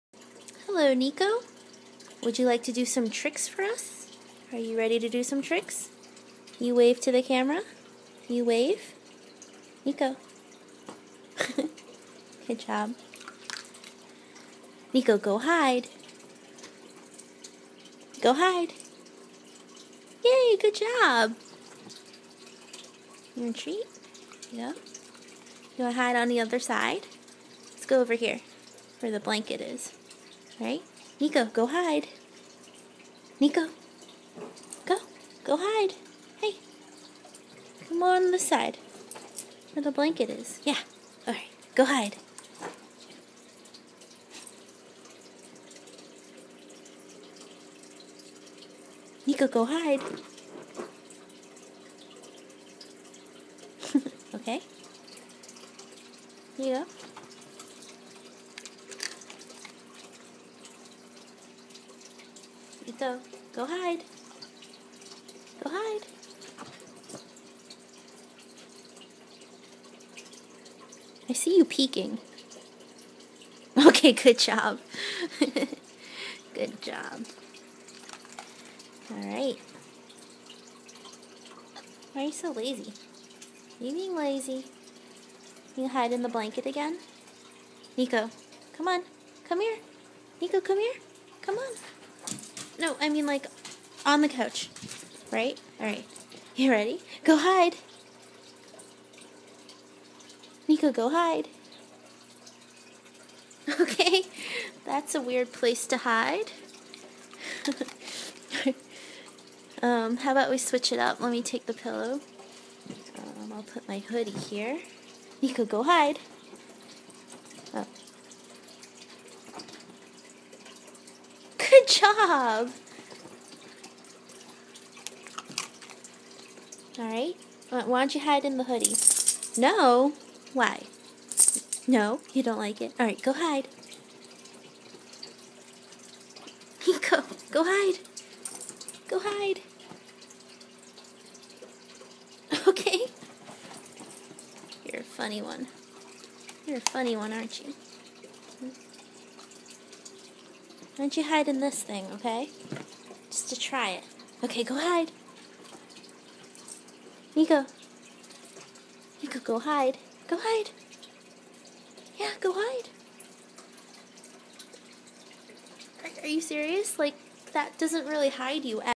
Dog learning to hide from predators
Niko the American Eskimo doing his go hide trick!

Thanks to everyone who shares this on their Twitter, Facebook, Pinterest, etc. it really means a lot to us :)